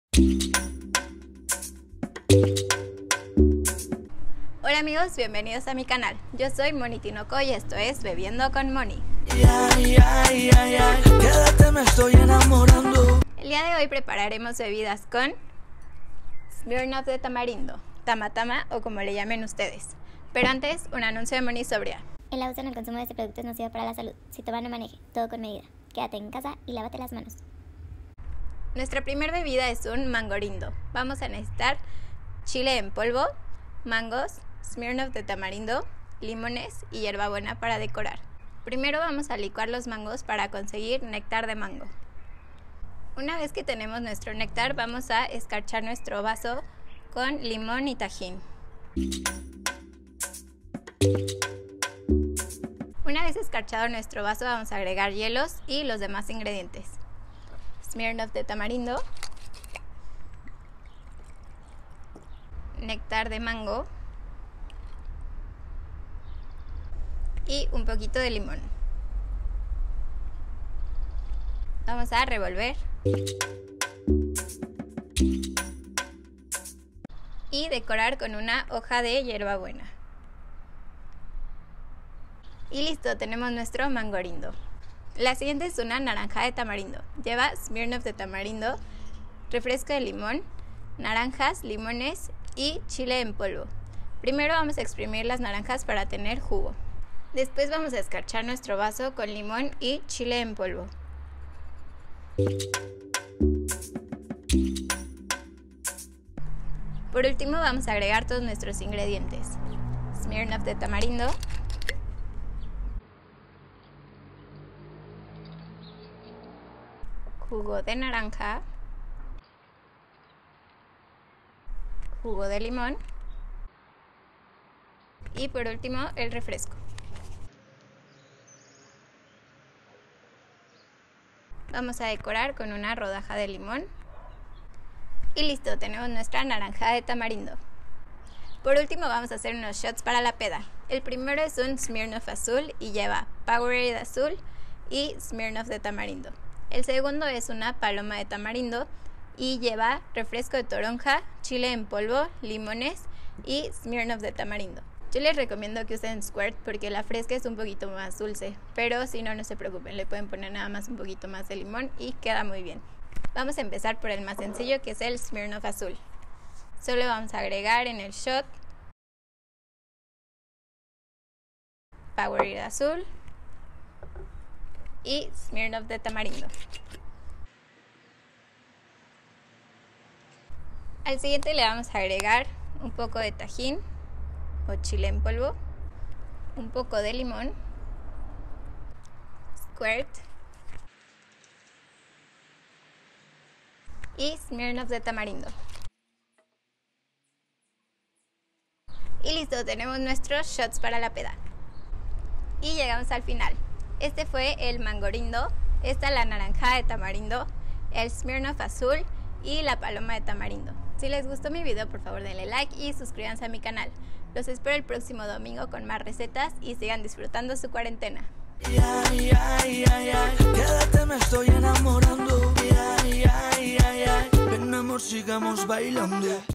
¿Cómo preparar tragos con Smirnoff de tamarindo?
¿Qué mejor para la cuarentena que unos tragos con vodka de tamarindo?

Ingredientes:

-Mangorindo

1 oz. Jugo de limón
2 oz. Nectar de mango
1 oz. Smirnoff de tamarindo
Hielos
Escarchar con limón y Tajín
Decorar con una hoja de hierbabuena

Opcional: 1 oz. Agua mineral

-Naranjada de tamarindo

1 oz. Jugo de limón
2 oz. Jugo de naranja
1 oz. Refresco de limón
1 oz. Smirnoff de tamarindo
Hielos
Decorar con una rodaja de limón

-Smirnoff Azul

1/2 oz. Powerade de moras
1/2 oz. Smirnoff de tamarindo

-Paloma de tamarindo

1 pizca Chile en polvo
1/4 oz. Jugo de limón
1/2 oz. Refresco de toronja
1/2 oz. Smirnoff de tamarindo

Tip:
El vodka de tamarindo no es agua de tamarindo